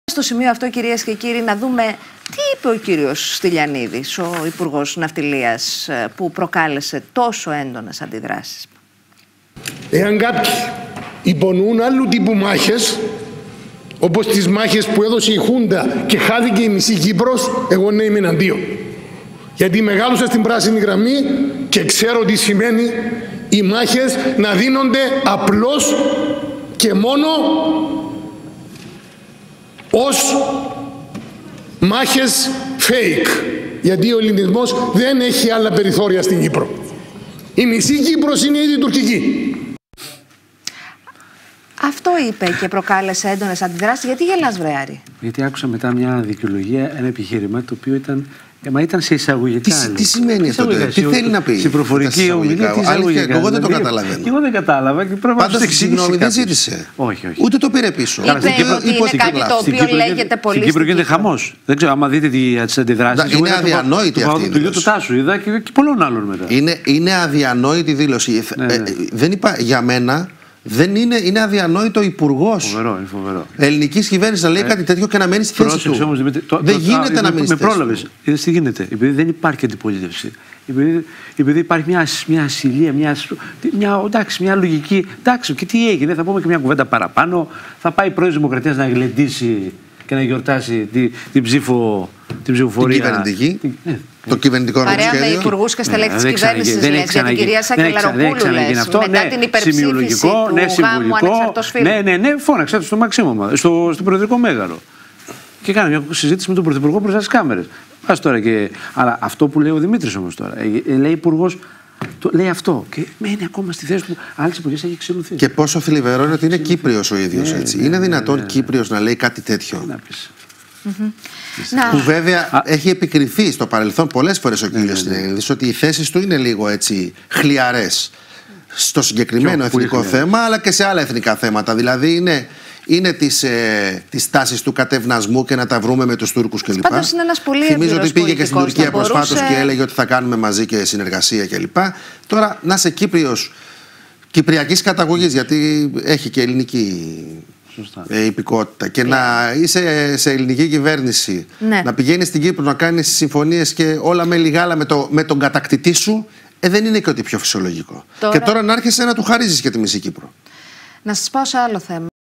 Χρήστος Στυλιανίδης: Αντιδράσεις για τη φράση «η μισή Κύπρος είναι ήδη τουρκική» | Kontra Channel
Έντονες αντιδράσεις των κομμάτων της αντιπολίτευσης προκάλεσε τα όσα είπε ο υπουργός Χρήστος Στυλιανίδης σε τοποθέτηση του στη Βουλή για την Κύπρο.

Ο κ. Στυλιανίδης ανέφερε μεταξύ άλλων ότι «ο ελληνισμός δεν έχει άλλα περιθώρια στην Κύπρο. Η μισή Κύπρος είναι ήδη τουρκική».

Η φράση αυτή προκάλεσε την έντονη αντίδραση της αντιπολίτευσης με τους βουλευτές να υποστηρίζουν ότι «ο υπουργός παραχώρησε την μισή Κύπρο στην Τουρκία».\

Μετά τις αντιδράσεις ο κ. Στυλιανίδης επανήλθε και τόνισε πως «η διατύπωση με ακριβώς τις ίδιες λέξεις έχει διατυπωθεί πολλές φορές στην κυπριακή Βουλή χωρίς να προκληθούν αντιδράσεις. Θεωρήθηκε από ορισμένους ότι εγώ παραχώρησα την μισή Κύπρο στην Τουρκία. Όσοι με ξέρουν ξέρουν και τους αγώνες μου για την ιδιαίτερη πατρίδα μου. Ήταν στα εισαγωγικά η φράση μου και πιστεύω ότι έτσι θα περάσει στα πρακτικά».